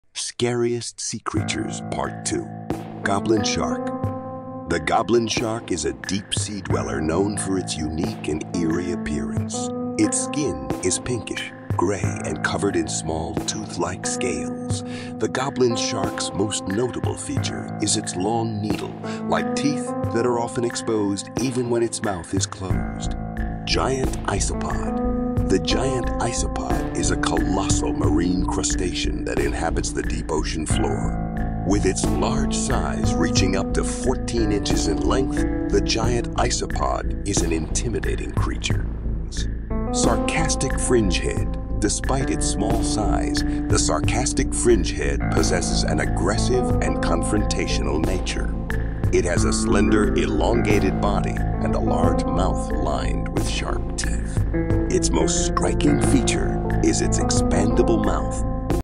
Scariest Sea Creatures Part 2.
Dive back into the depths of the ocean to encounter the Scariest Deep Sea Creatures in Part Two of this captivating YouTube Shorts series! Brace yourself for another heart-pounding adventure as we unveil more bone-chilling wonders lurking beneath the surface.